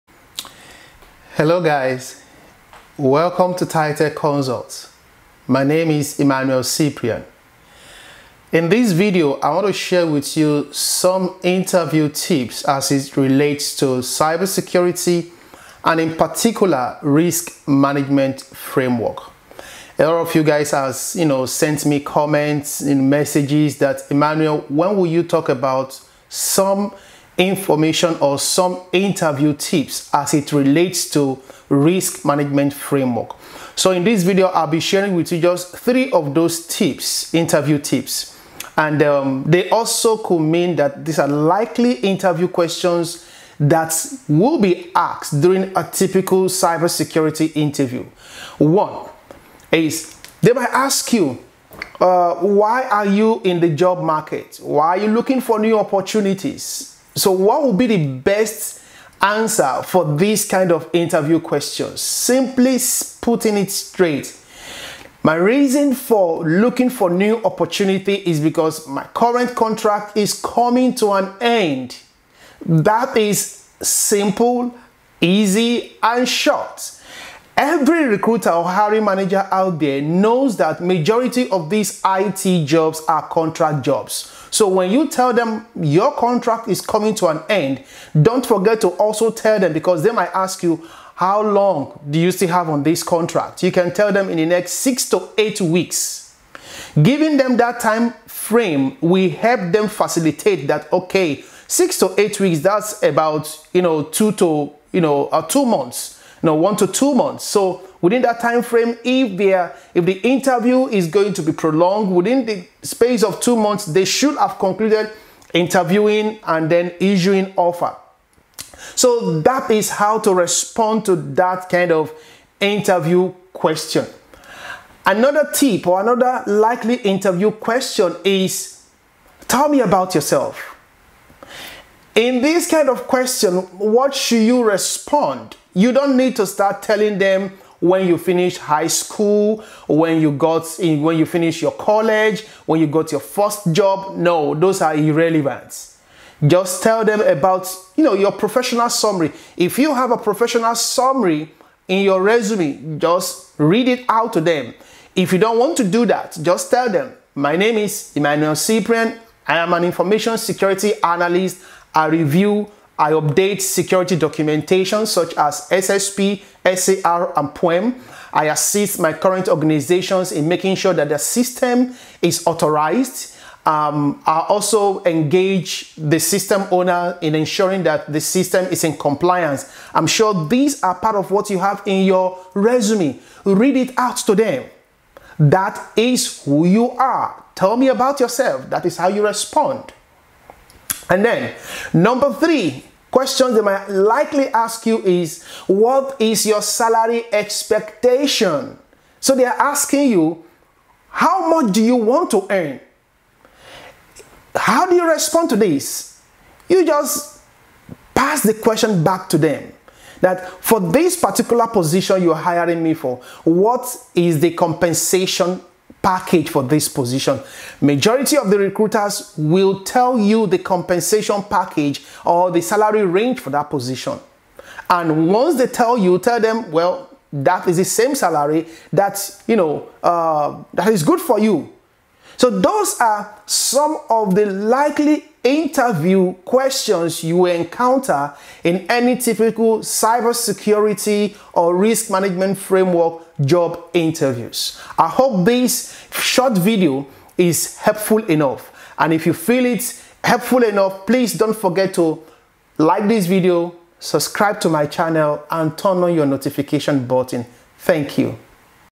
Risk Management Framework/Cyber Security Interview Questions & Answers
This video explains some of the likely Cyber Security/RMF job interview questions and answers.